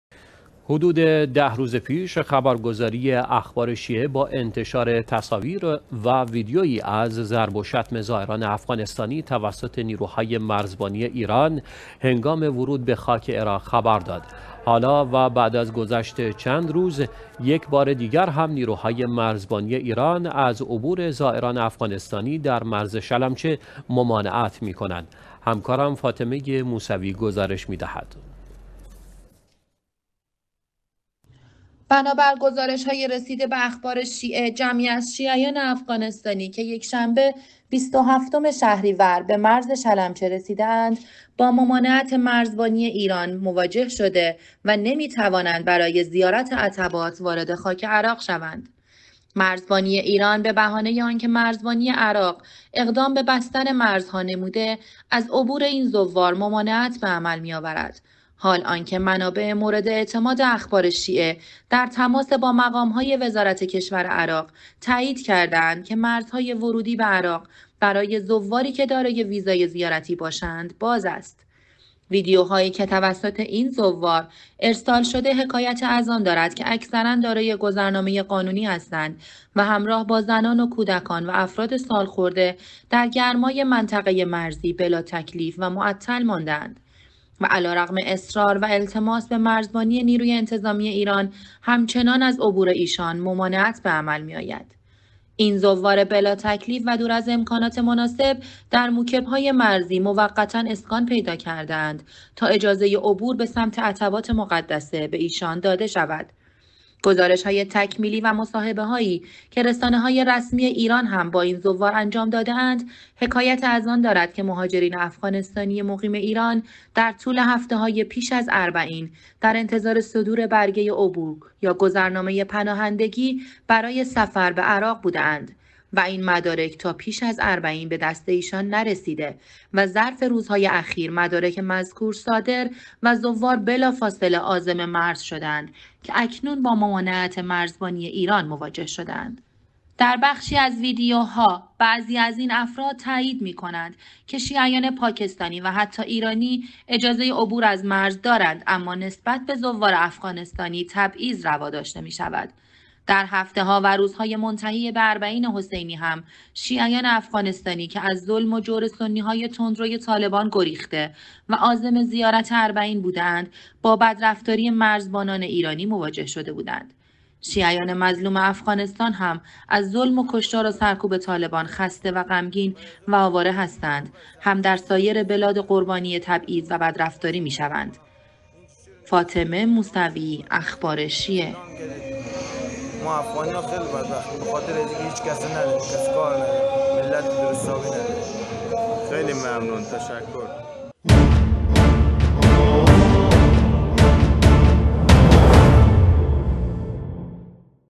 از ضرب و شتم تا ممانعت مرزبانی ایران از عبور زائران افغانستانی - جزئیات بیشتر در گزارش فاطمه موسو ی
حدود ۱۰ روز پیش، خبرگزاری اخبار شیعه با انتشار تصاویر و ویدئویی از ضرب و شتم زائران افغانستانی توسط نیروهای مرزبانی ایران، هنگام ورود به خاک عراق خبر داد.

حالا و بعد از گذشت چند روز، یک بار دیگر هم نیروهای مرزبانی ایران از عبور زائران افغانستانی در مرز شلمچه ممانعت می کنند.

بنابر گزارش های رسیده به اخبار شیعه، جمعی از شیعیان افغانستانی که یکشنبه، 27 شهریور به مرز شلمچه رسیده اند، با ممانعت مرزبانی ایران مواجه شده و نمی توانند برای زیارت عتبات وارد خاک عراق شوند.

مرزبانی ایران به بهانه آنکه مرزبانی عراق اقدام به بستن مرزها نموده، از عبور این زوار ممانعت بعمل می آورد، حال آنکه منابع مورد اعتماد اخبار شیعه در تماس با مقام های وزارت کشور عراق تایید کردند که مرزهای ورودی به عراق برای زواری که دارای ویزای زیارتی باشند، باز است.

ویدئوهایی که توسط این زوار ارسال شده، حکایت از آن دارد که اکثرا دارای گذرنامه قانونی هستند و همراه با زنان و کودکان و افراد سالخورده در گرمای منطقه مرزی بلاتکلیف و معطل مانده اند و علیرغم اصرار و التماس به مرزبانی نیروی انتظامی ایران، همچنان از عبور ایشان ممانعت بعمل می آید.

این زوار بلاتکلیف و دور از امکانات زیست مناسب در موکب های مرزی موقتا اسکان پیدا کرده اند تا اجازه عبور به سمت عتبات مقدسه به ایشان داده شود.

گزارش های تکمیلی و مصاحبه هایی که رسانه های رسمی ایران هم با این زوار انجام داده اند حکایت از آن دارد که مهاجرین افغانستانی مقیم ایران در طول هفته های پیش از اربعین در انتظار صدور برگه عبور یا گذرنامه پناهندگی برای سفر به عراق بوده اند و این مدارک تا پیش از اربعین بدست ایشان نرسیده و ظرف روزهای اخیر مدارک مذکور صادر و زوار بلافاصله عازم مرز شده اند که اکنون با ممانعت مرزبانی ایران مواجه شده اند.

در بخشی از ویدئوها، بعضی از این افراد تایید می کنند که شیعیان پاکستانی و حتی ایرانی اجازه عبور از مرز دارند اما نسبت به زوار افغانستانی تبعیض روا داشته می شود.

در هفته ها و روزهای منتهی به اربعین حسینی هم، شیعیان افغانستانی که از ظلم و جور سنی های تندروی طالبان گریخته و عازم زیارت اربعین بودند با بدرفتاری مرزبانان ایرانی مواجه شده بودند.

شیعیان مظلوم افغانستان هم از ظلم و کشتار و سرکوب طالبان خسته و غمگین و آواره هستند هم در سایر بلاد قربانی تبعیض و بدرفتاری می باشند.